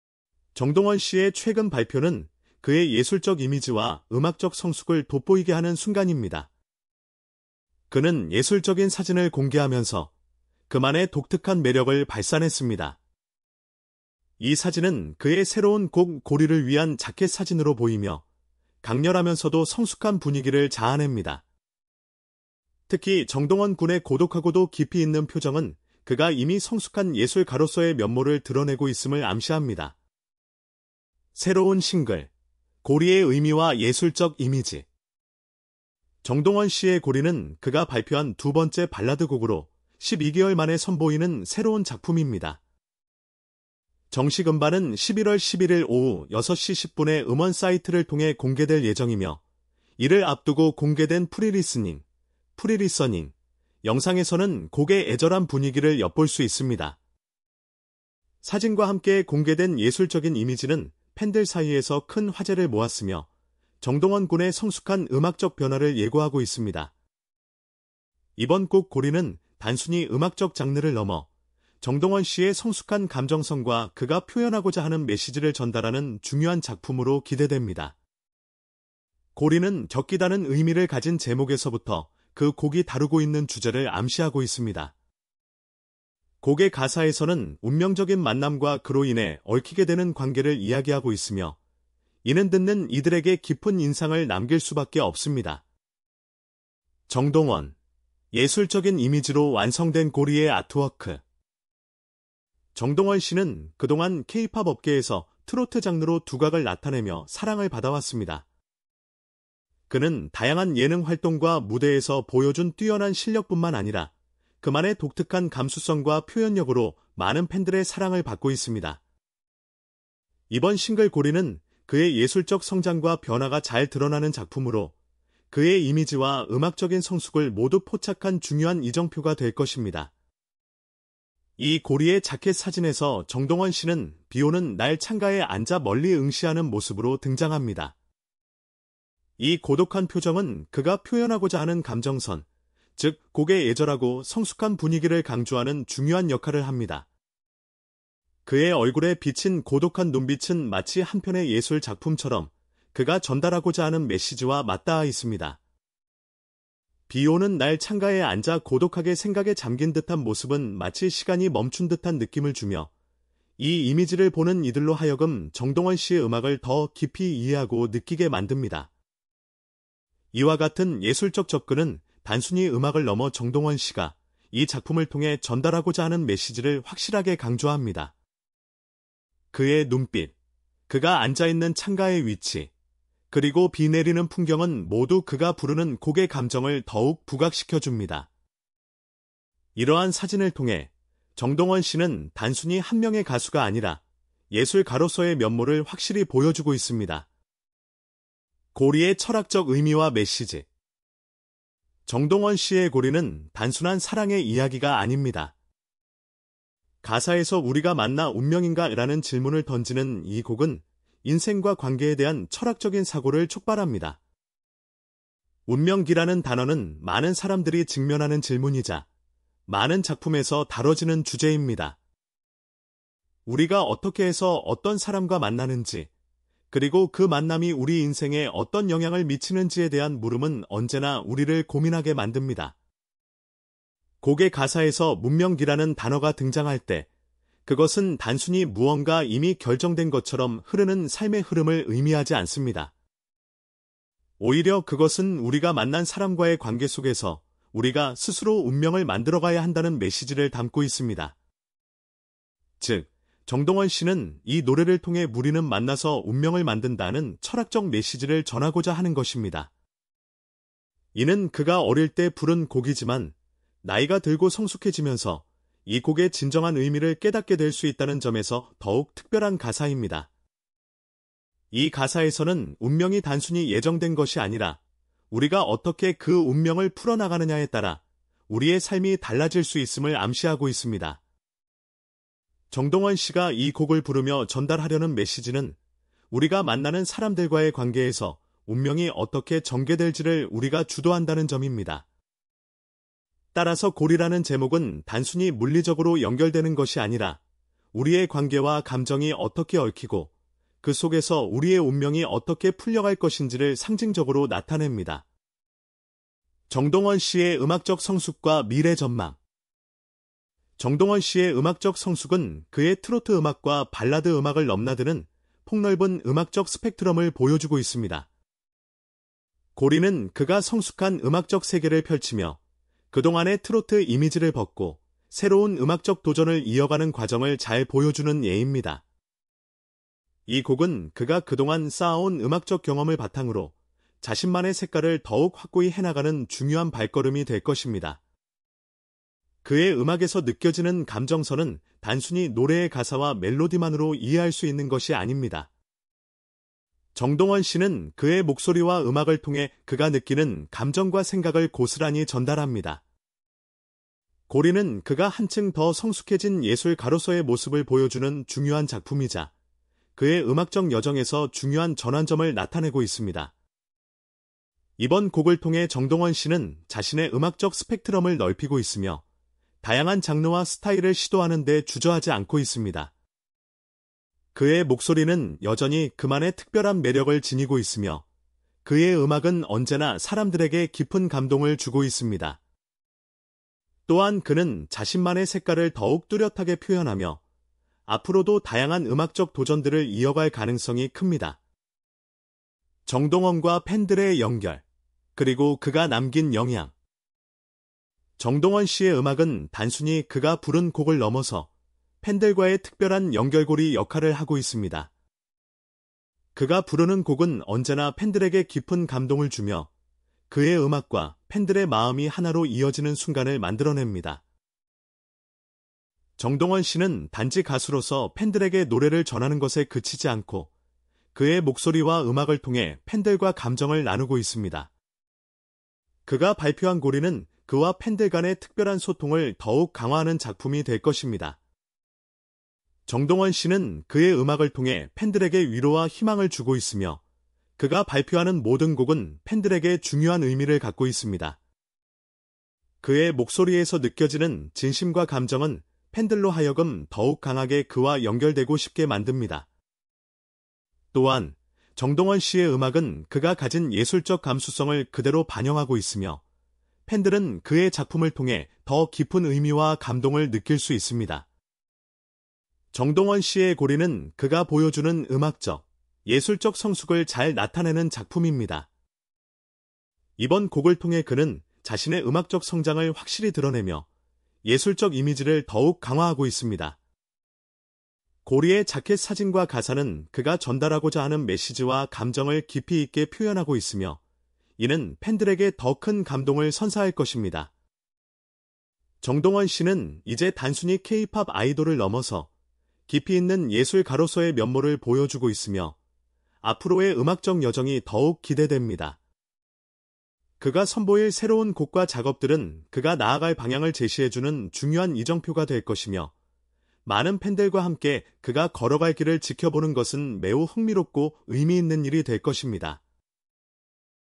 정동원, ‘고리’ 예술 사진 공개! 강렬한 인상 남긴 독특한 작품!
채널을 구독하고 벨을 눌러 최신 영상을 시청하세요!